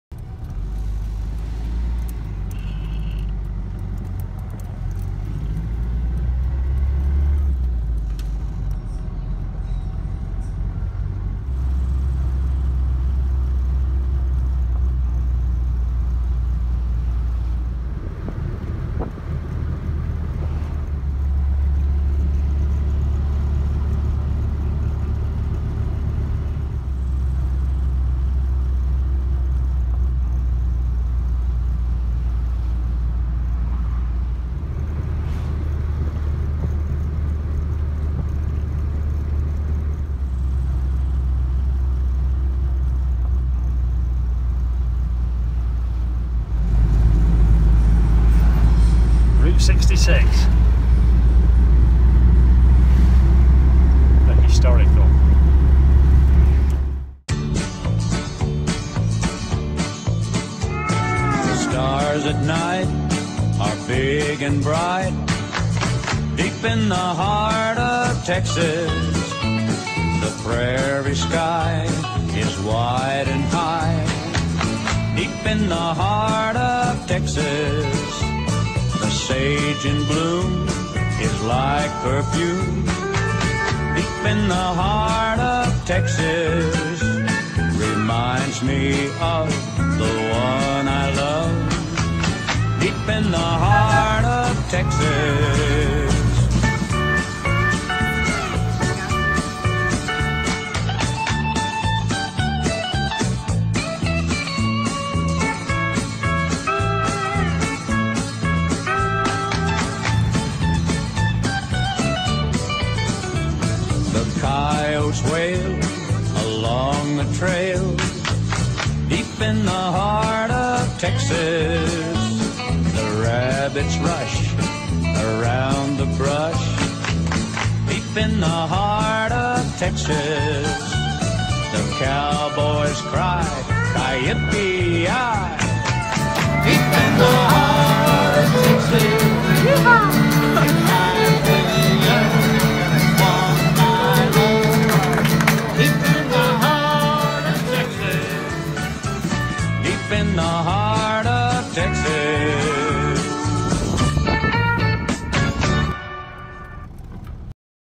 Historic Route 66 - In a Mini
Home Sick today and unable to do much of anything, I may have to pull this video down at some point, but thought I would give a TEXAS size shout out to a new and dear Mini friend, Pete Raymond, who came for a visit. He was intrigued with America's Hwy Route 66, so we could not let hm come this far and not travel the road. in nothing less than a MINI... Anyone that comes to Texas, must stop in and we'll treat you to the worlds best BBQ!!